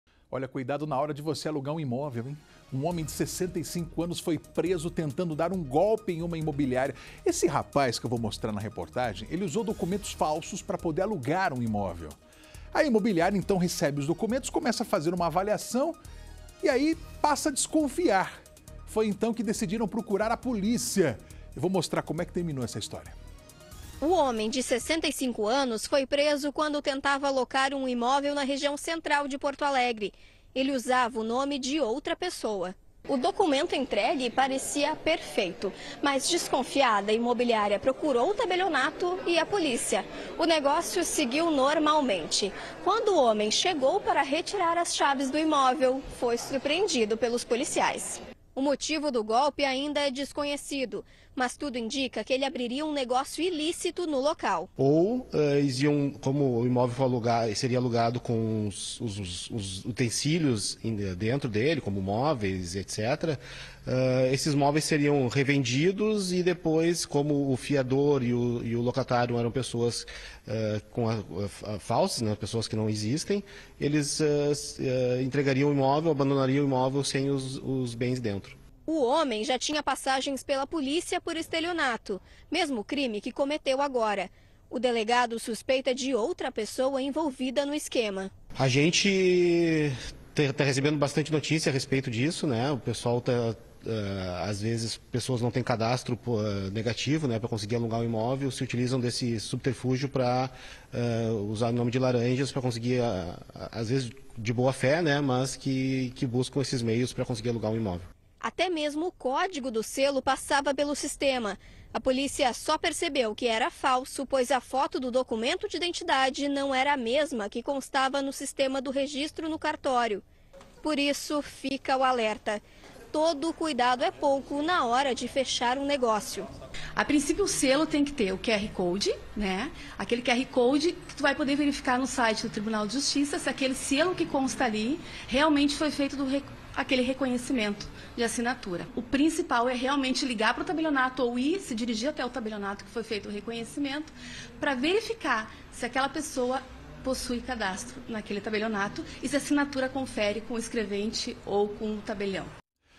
Homem é preso ao tentar alugar imóvel com documento falso
Um homem de 65 anos foi preso tentando usar documentos falsos para locar um imóvel na região de Porto Alegre (RS). Desconfiada, a imobiliária procurou a polícia e denunciou o criminoso, que foi preso em flagrante. Para assistir ao conteúdo na íntegra, acesse o PlayPlus.com